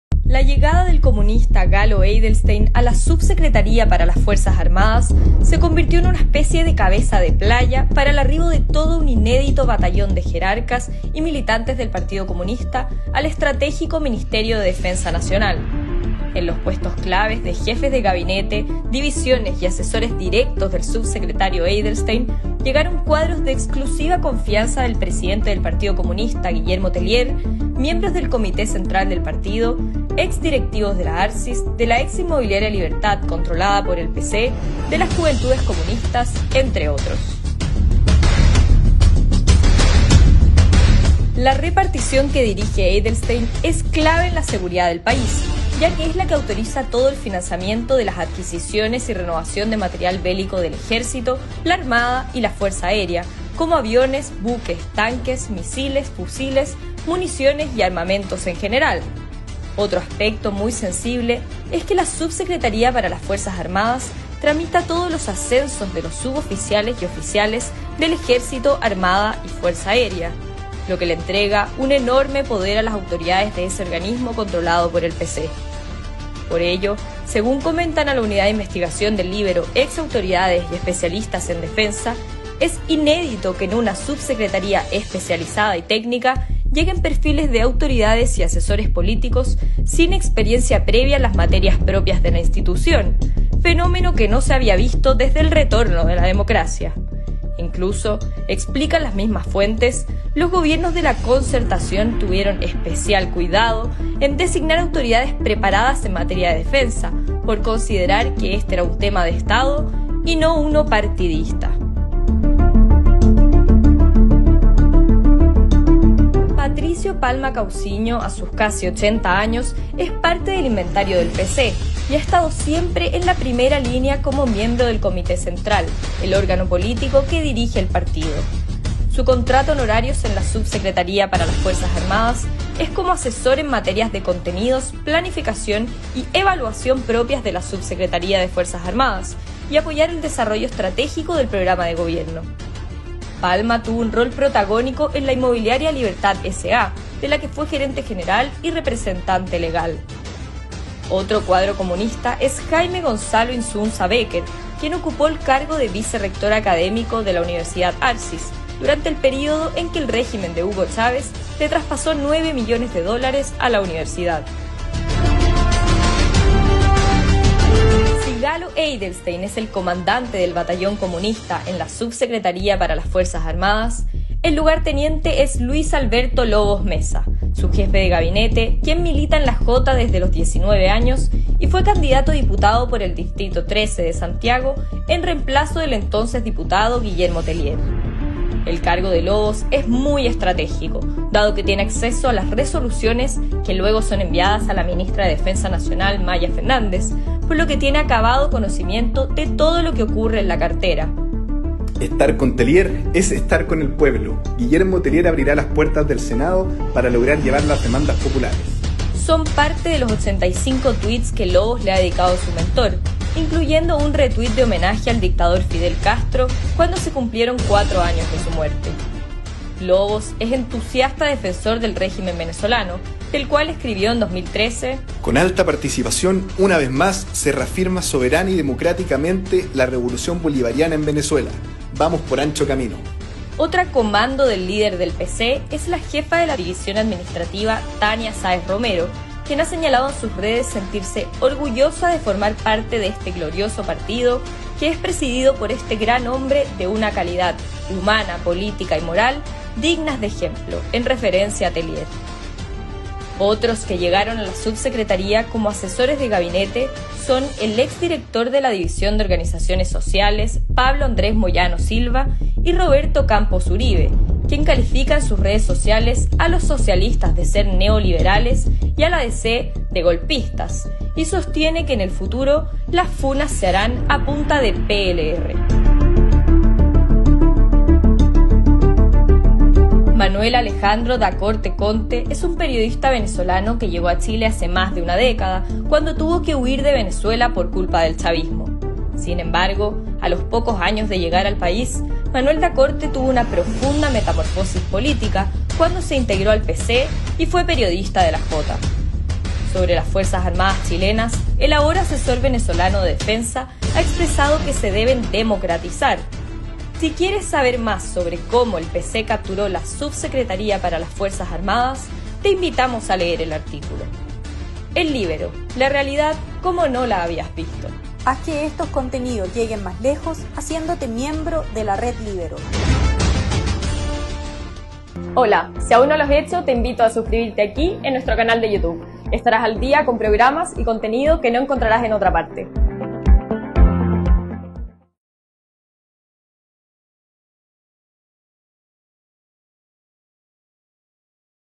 Llegada del partido comunista al ministerio de defensa nacional. Nefasto para el país!!!!
Llegada del partido comunista al ministerio de defensa nacional. Nefasto para el país!Nada que hacer el comunismo ahí!Estudios, trabajo y perfeccionamiento continuos.